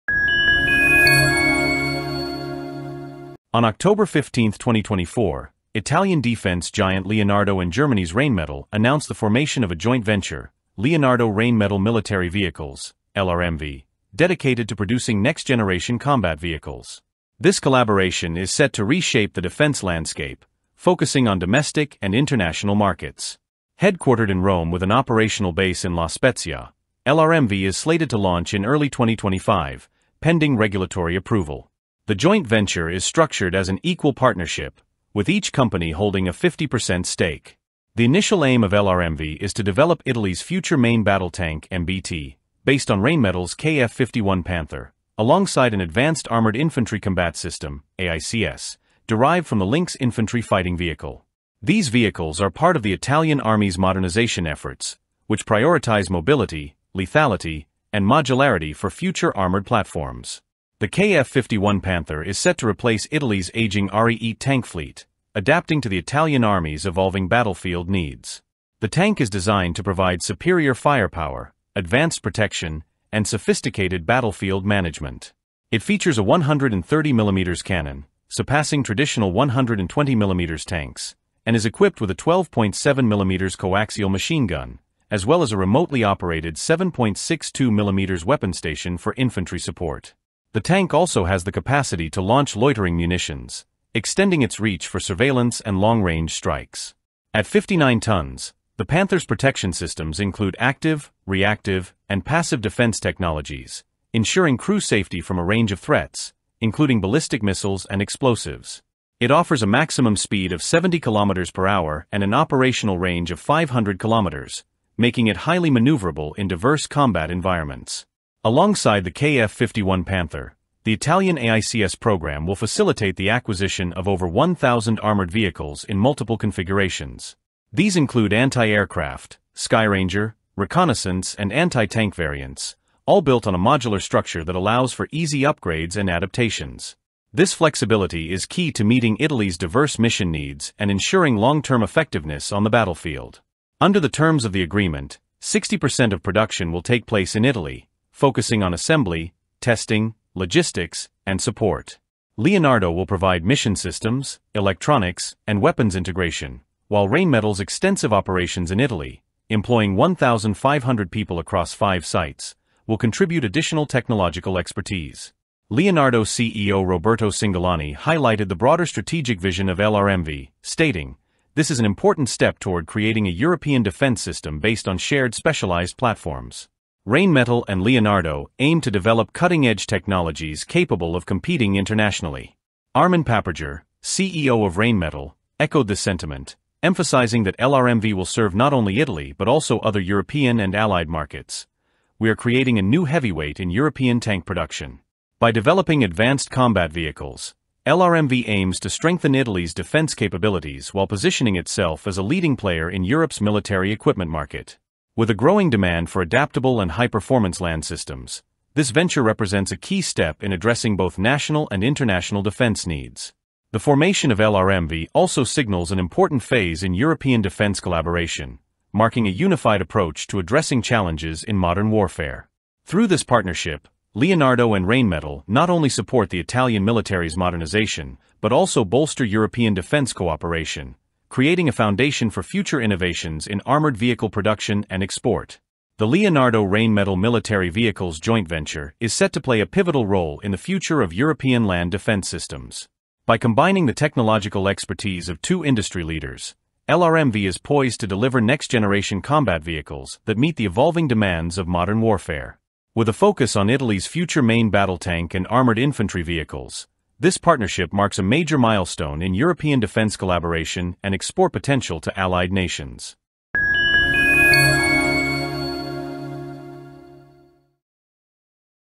Leonardo and Rheinmetall Join Forces to Develop Italy's Future Main Battle Tank
Leonardo and Rheinmetall have announced the formation of a new joint venture, Leonardo Rheinmetall Military Vehicles (LRMV), dedicated to developing next-generation combat vehicles. This strategic partnership aims to produce Italy’s future main battle tank (MBT) based on the KF51 Panther, as well as an advanced Armoured Infantry Combat System (AICS) derived from the Lynx infantry fighting vehicle. LRMV’s primary goal is to support Italy’s military modernization program by delivering armored vehicles with enhanced mobility, modularity, and lethality. The KF51 Panther is designed to replace the Ariete tank, offering cutting-edge protection and firepower, including a 130 mm cannon and advanced defense systems. With a focus on European defense collaboration and export potential, LRMV plans to strengthen Italy's position in the global defense market. Production activities will be based in Italy, ensuring local economic benefits and technological innovation.